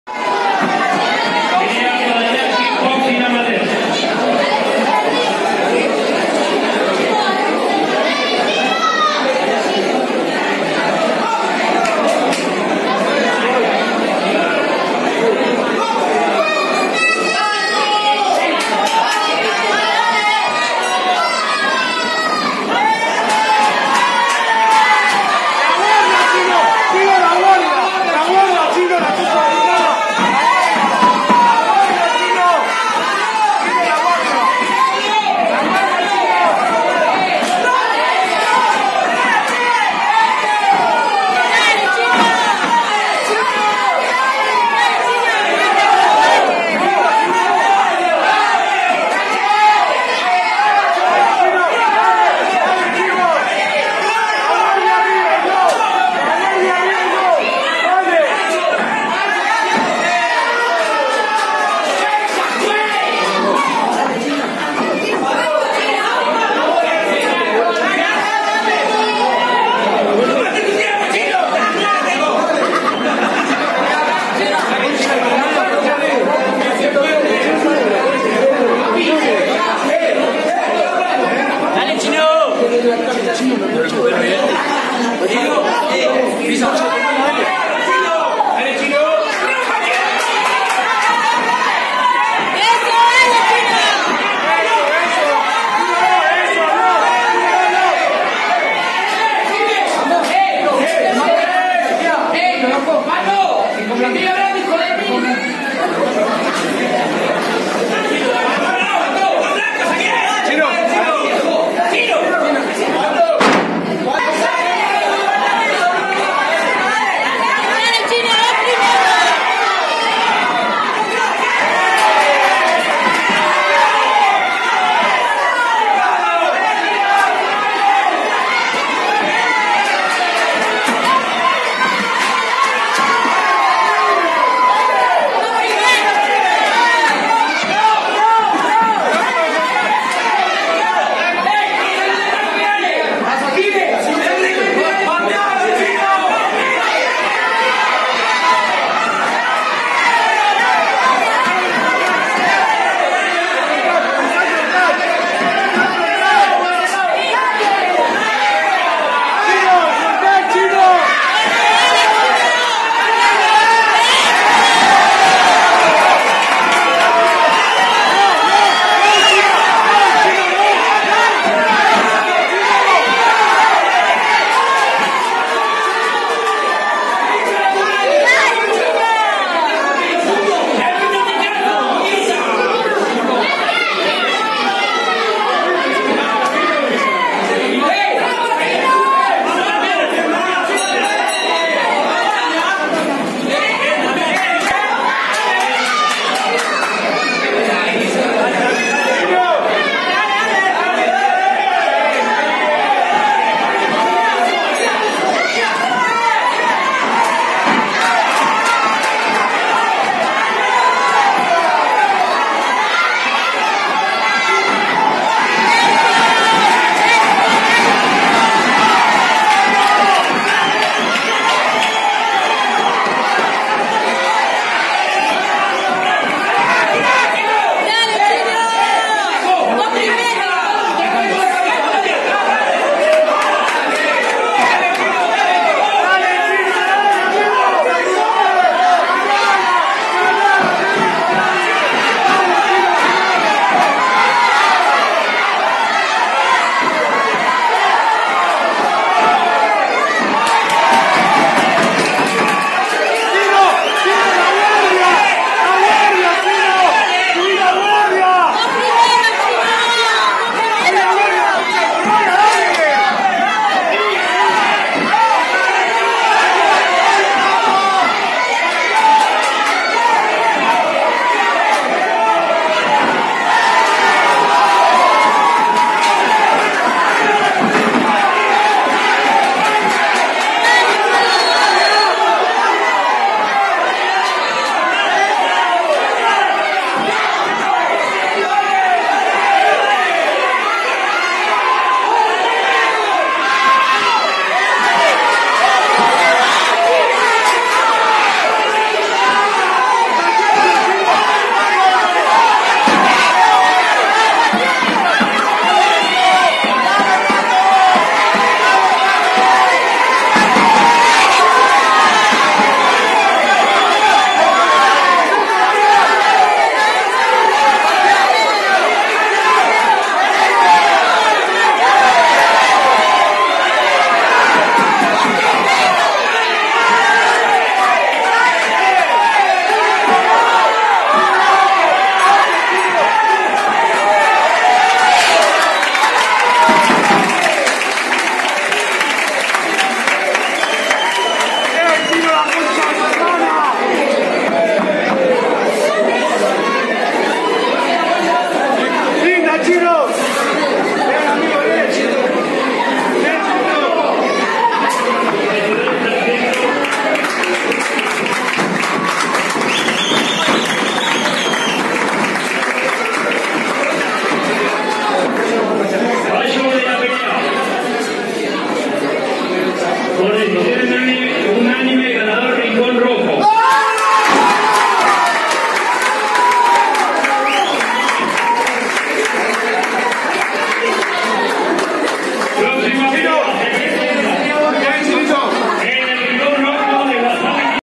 🥊 Damián Alderete Vs Jonathan Pereyra - KICK BOXING - AMATEUR - Campeones Del Pueblo
🥊 Campeones Del Pueblo - Siempre Humilde

🥊Querés qué filme y suba tú pelea a YouTube? Háblame ‼